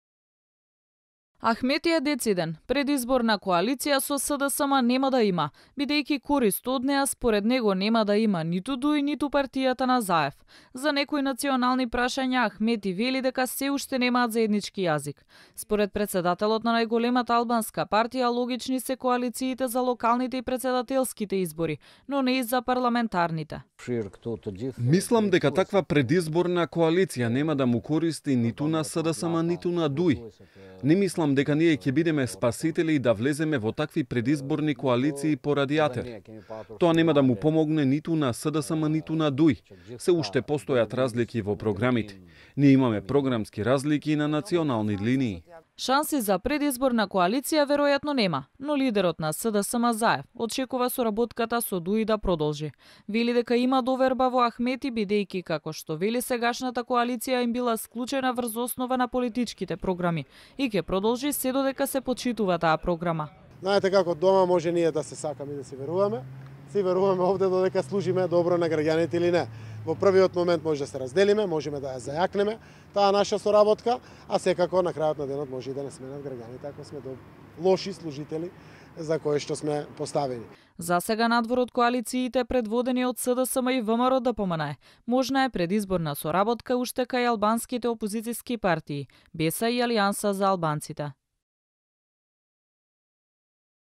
Нема предизборна коалиција ДУИ-СДСМ
Ахмети е дециден – предизборна колаиција со СДСМ нема да има, бидејќи корист од неа, според него, нема да има ниту ДУИ ниту партијата на Заев.